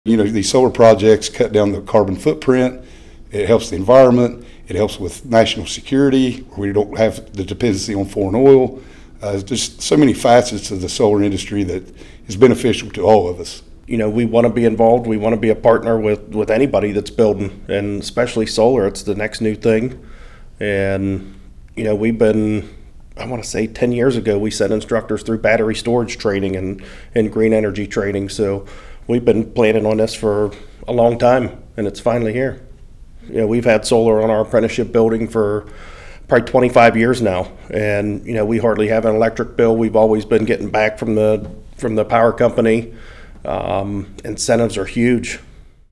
IBEW leaders are ready to build solar in Ohio
.@ibew leaders in Ohio have been ready to build the next phase of solar energy development across the state. They support House Bill 197 and know that a community solar pilot project will bring jobs and energy independence to the Buckeye State.